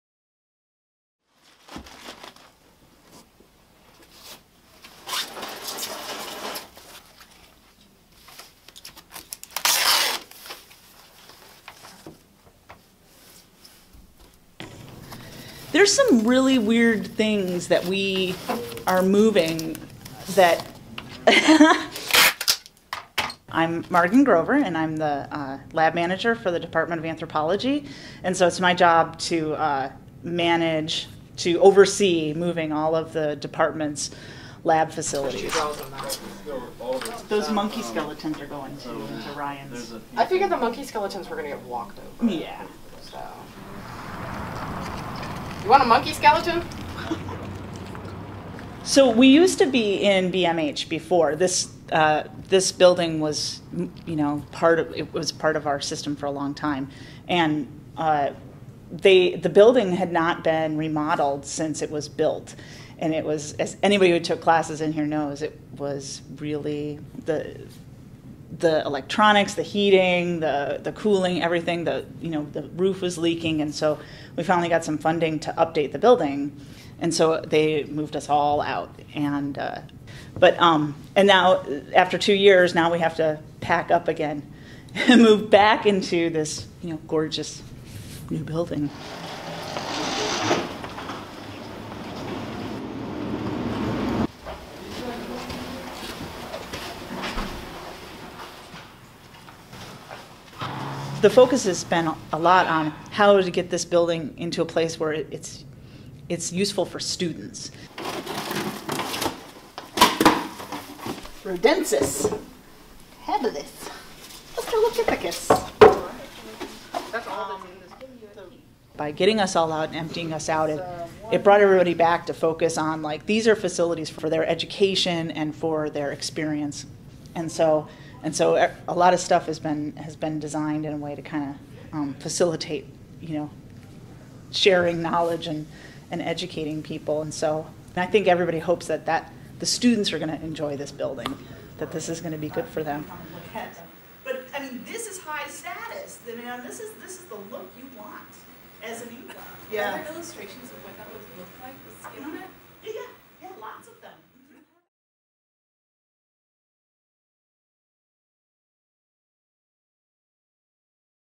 UAA Anthropology makes move back to Beatrice G. McDonald Hall
After two years in the Professional Studies Building with a temporary lab downtown, UAA's Anthropology Department is returning to the newly-renovated Beatrice G. McDonald Hall.

By James R. Evans

Photo Editor
The Northern Light